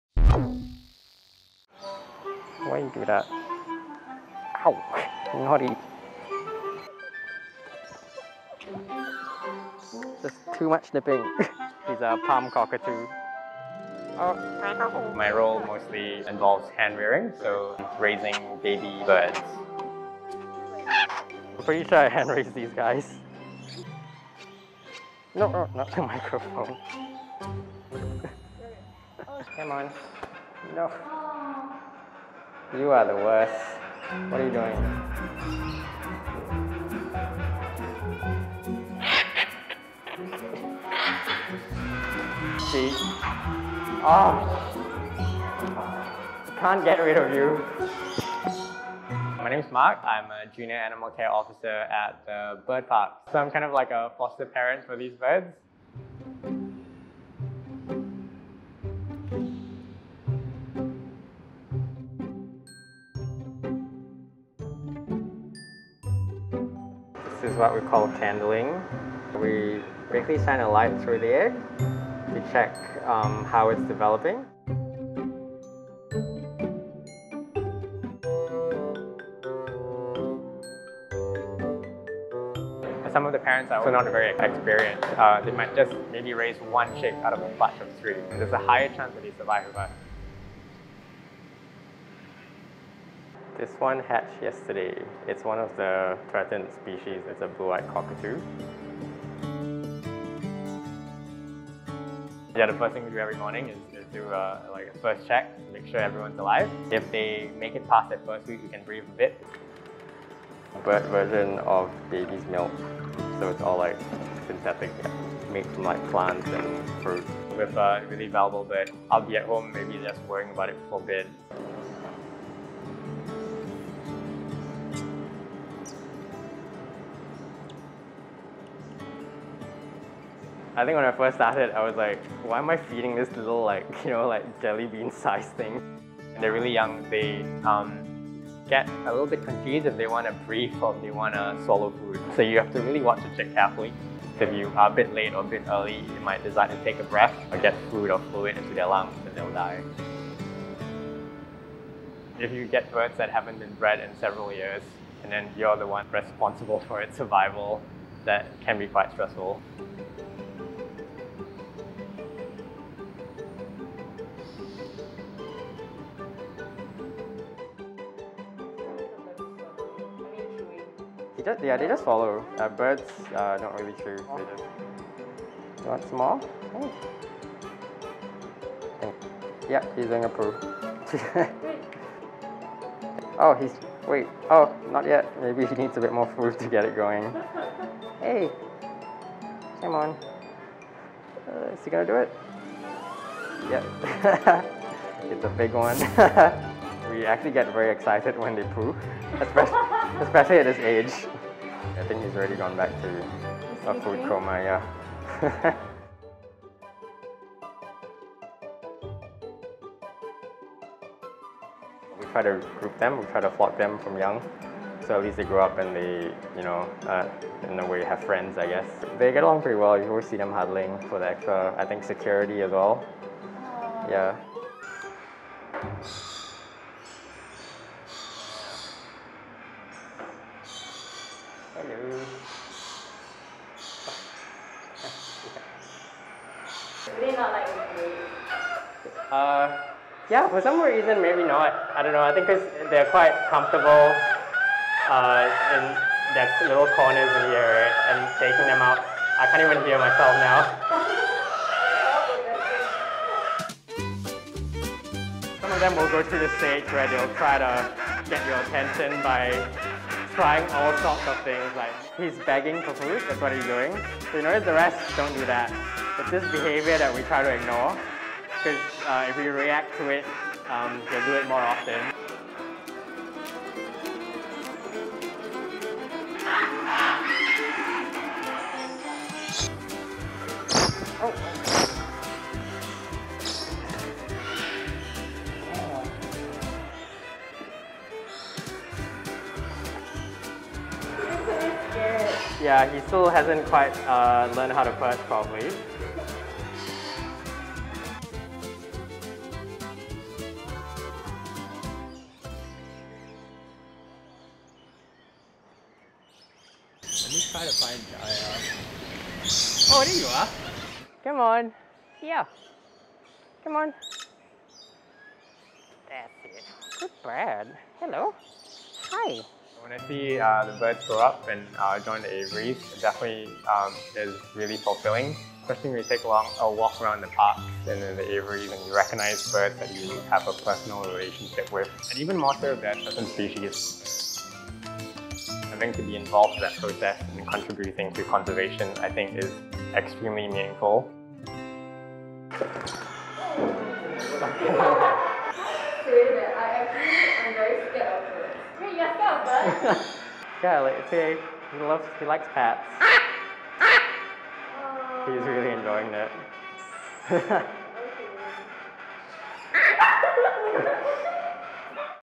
Bird Park's 'Foster Daddy' To Baby Endangered Birds
Baby birds like macaws, lorikeets, birds of paradise and cockatoos are hand-raised by a team of four "foster parents" at Singapore's Jurong Bird Park.
From when they were eggs in an incubator, to hatchlings being hand-fed, to fledglings learning to fly, these endangered birds have their (almost) papa bird, Mark Rusli, to thank.